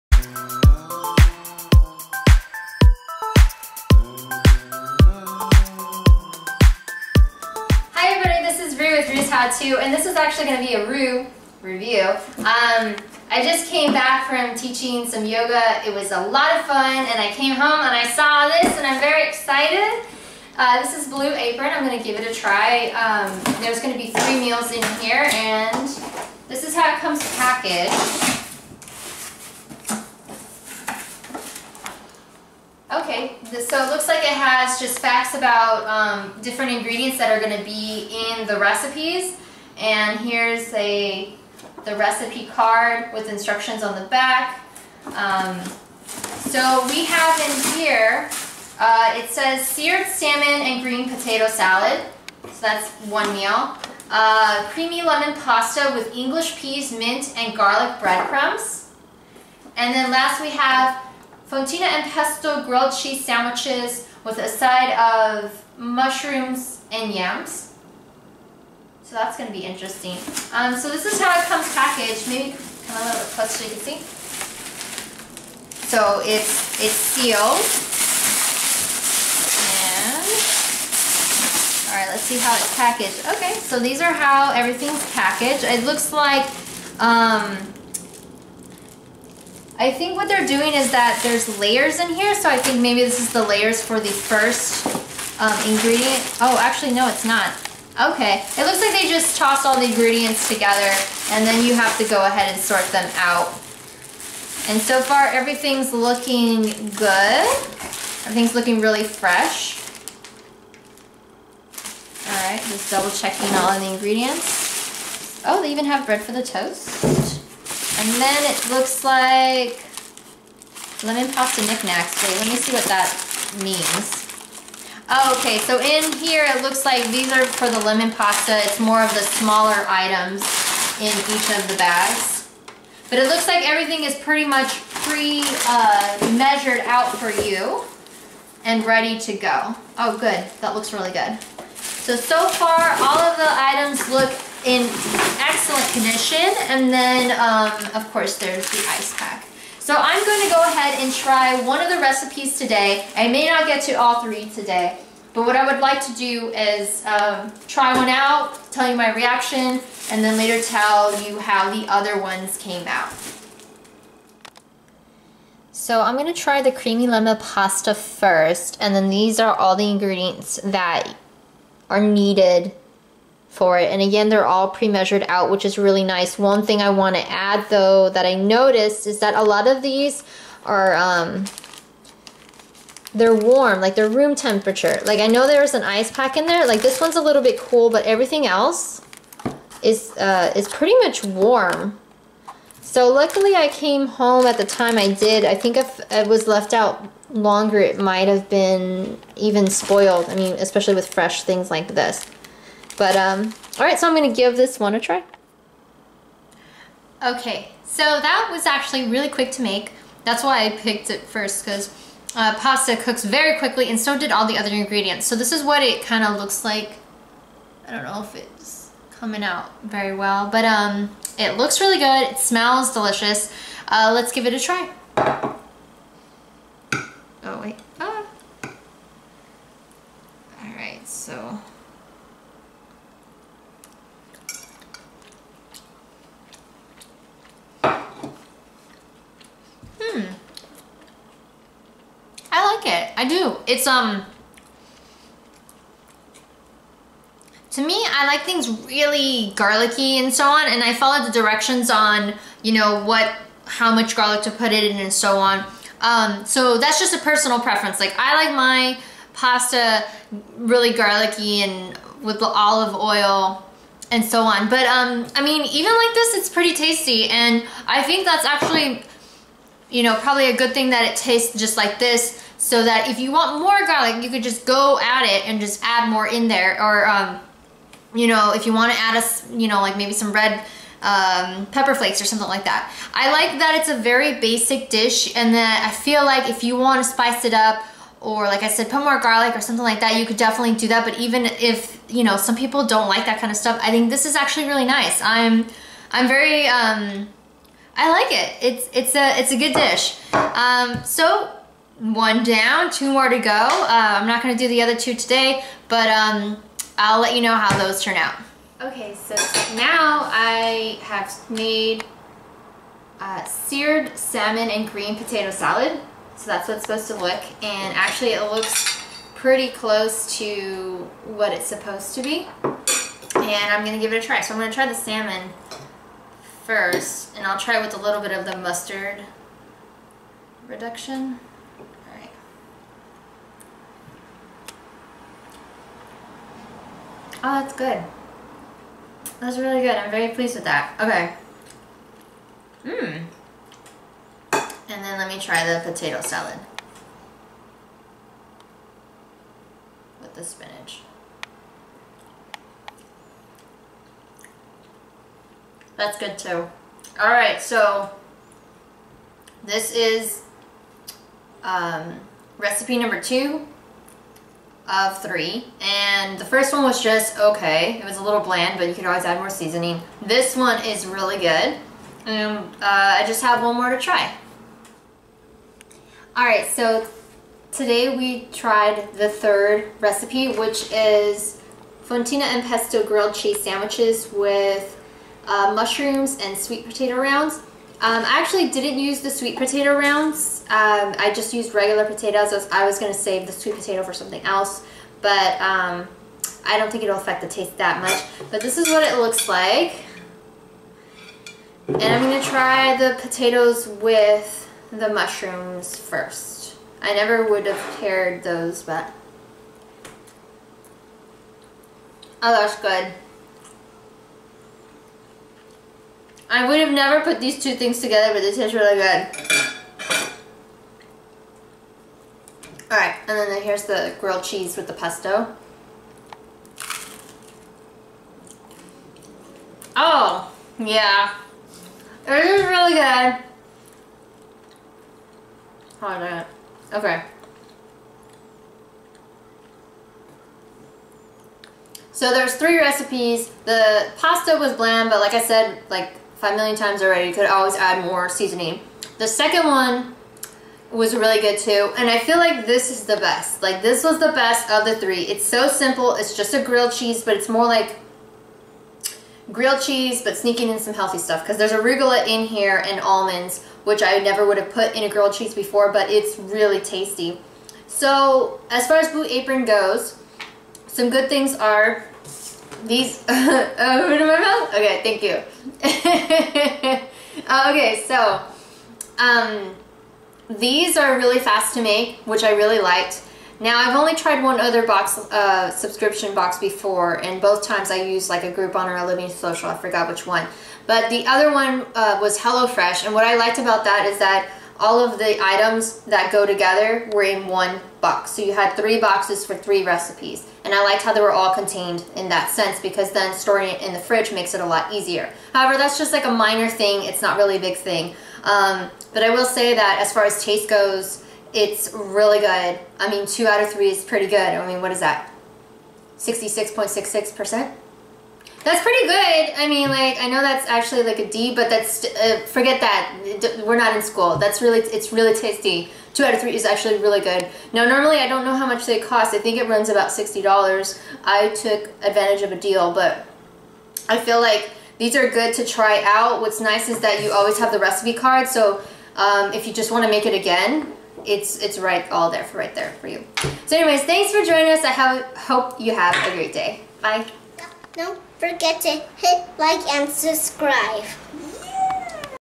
Roo's How To - Review Edition- Blue Apron
Hi amazing people!! Ok, so this video was originally created in April and for some reason, I did not get around to uploading it until now. I went into detail about how dish 1 tastes, however I feel I should expand a bit on how dish 2 and 3 tasted:

Dish 2: The mustard reduction added a nice touch of sweetness and crunchiness to the salmon. I hate potato salad, however, this was very tasty.  It had seasoned greens mixed in with the potato and was super delicious and not heavy on mayo.

Dish 3: Fontina is a tasty, savory cheese that melts nicely.  The Pesto was made of seasoned arugula and almonds and it really kicked it up from a kid sandwich to an amazing grilled cheese for adults.  I wanted to save the sweet potato for another dish, but regular potatoes made a nice substitution.  The side dish consisted of seasoned greens, potatoes and mushrooms and together it was an amazing umami dish! Loved it!

 Blue Apron did a great job of delivering fast and tasty recipes that were easy to cook.  It was fun to try and what I liked best is that you get recipe cards so you can create the same dish again in the future. I want to add that uou also don't have to receive a box each week; they allow you to skip weeks and will only charge you for the weeks when you do receive a box.

Hope you all have an awesome day!

**Shout out to Marcelo Robles for the AMAZING intro song.  You are amazing! **